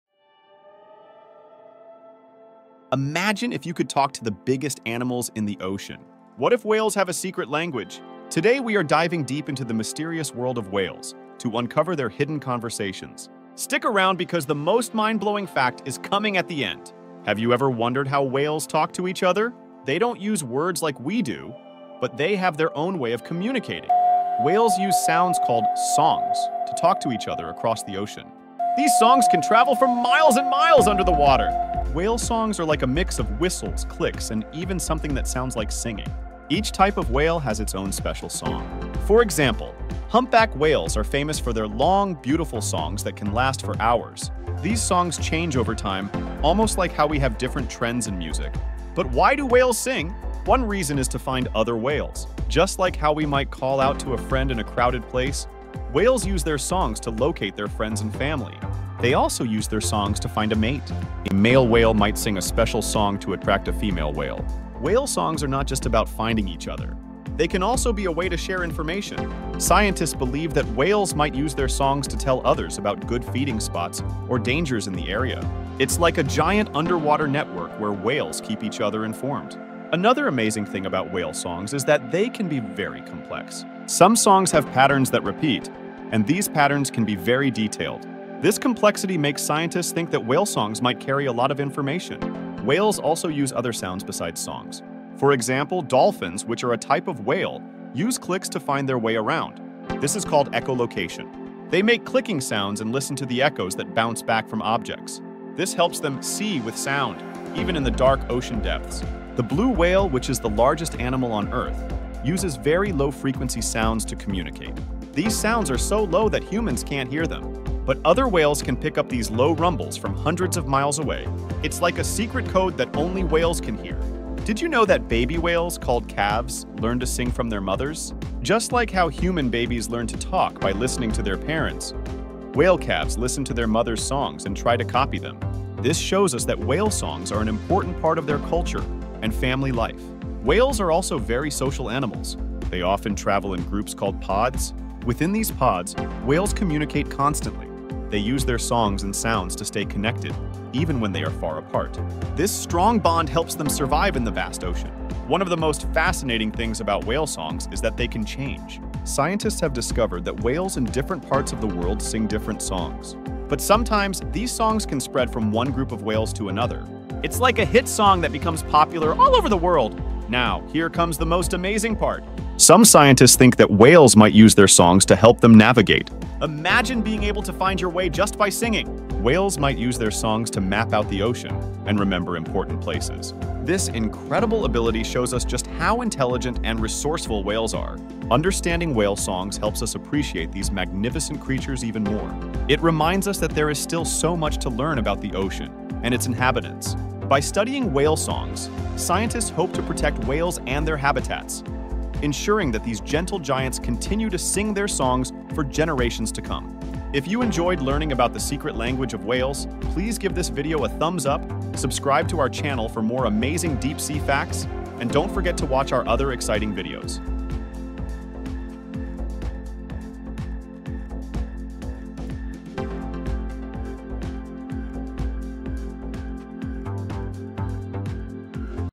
Discovering the Secret Language of Whales | Deep Sea Facts
Dive into the mysterious world of whales as we explore their fascinating secret language. Whales communicate through intricate songs that travel across vast oceans, using sound waves to find each other, share information, and navigate the deep sea. Join us on an educational journey to uncover how these majestic creatures use their unique abilities to survive and thrive.